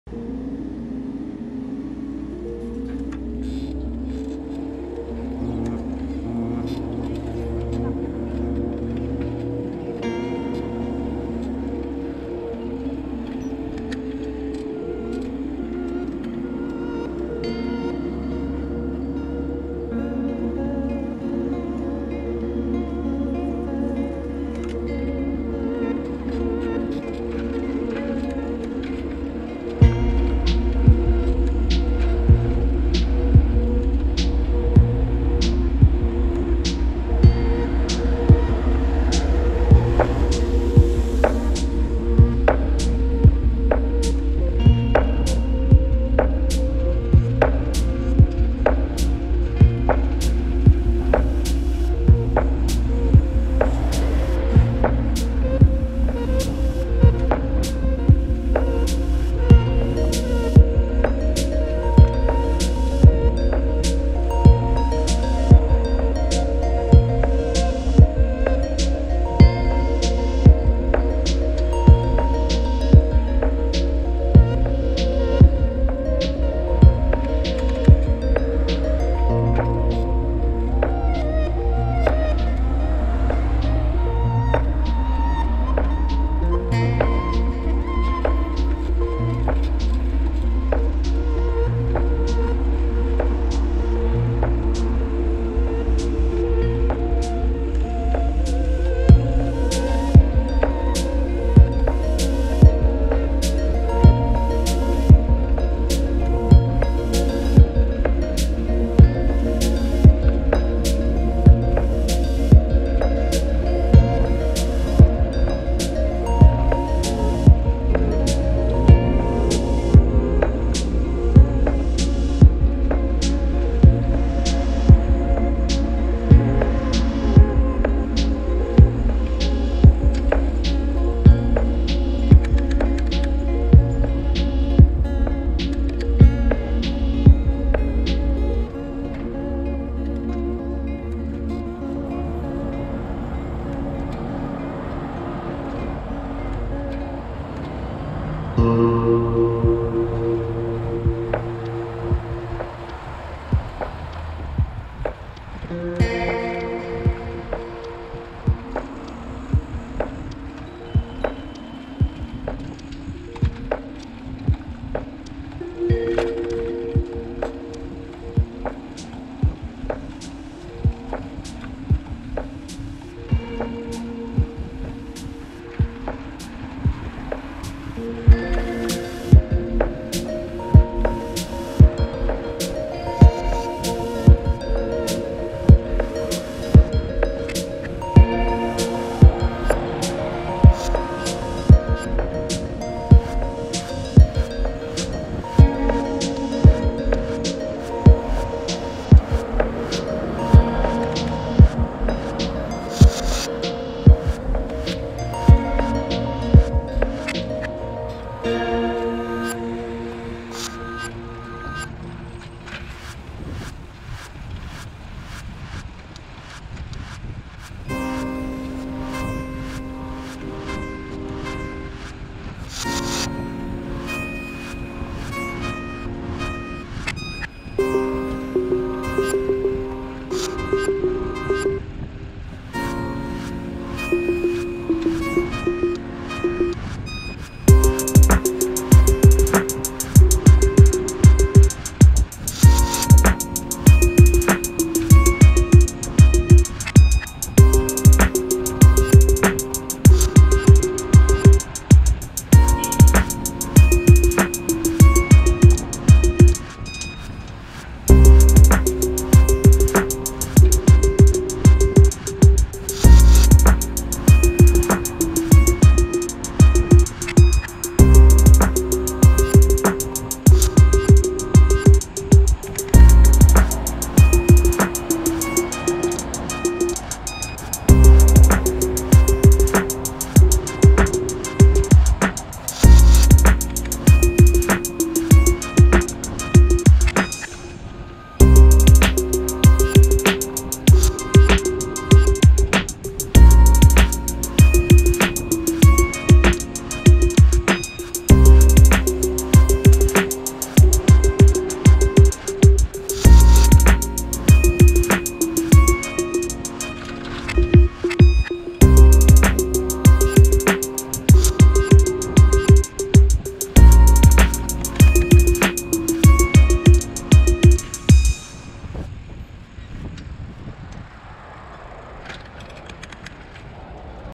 Copenhagen Cycling: Autumn Morning Rush Hour
Morning office run down Nørrebrogade in early autumn.